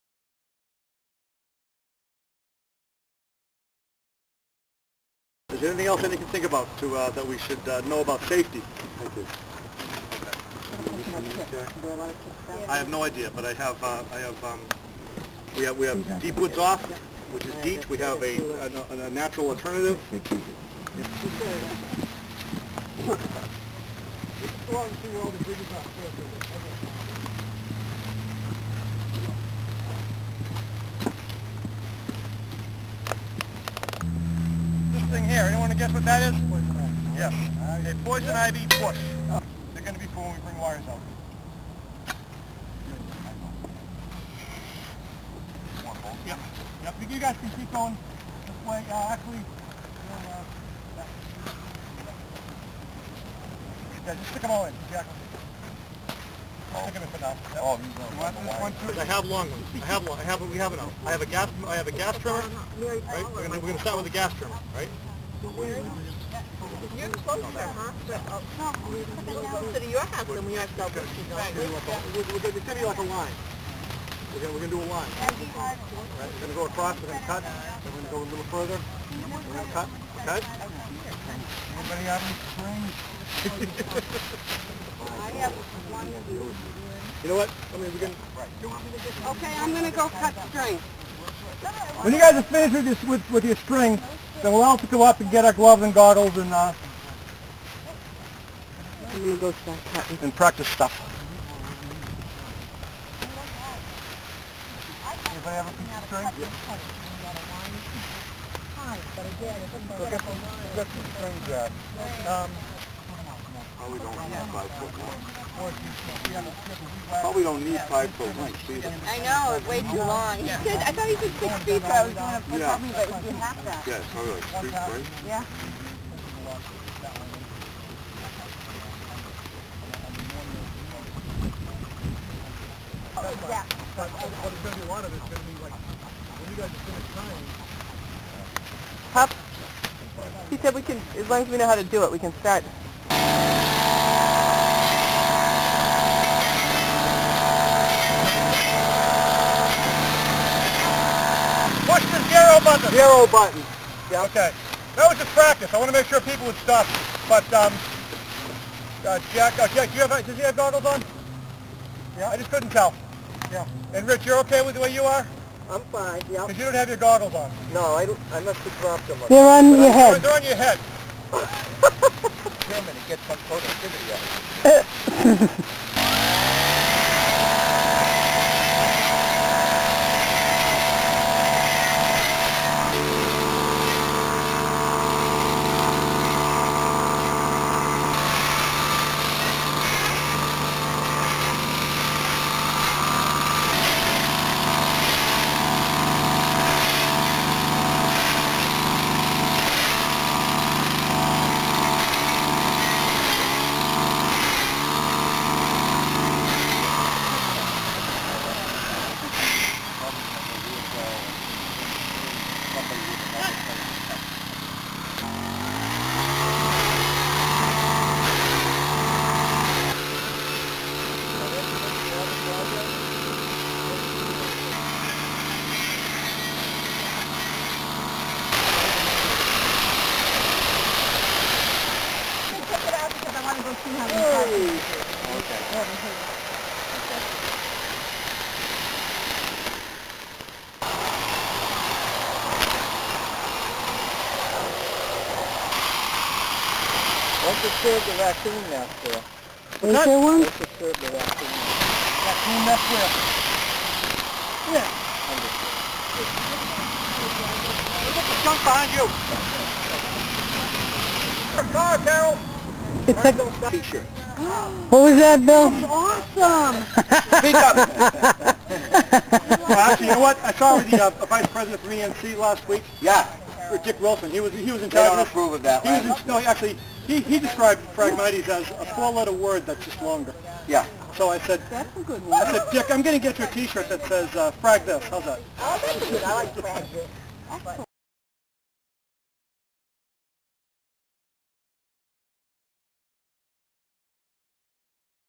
The Phrag Mighties, Quincy, MA
An environmental experiment in removing phragmites from the marsh.  Not for the faint of heart!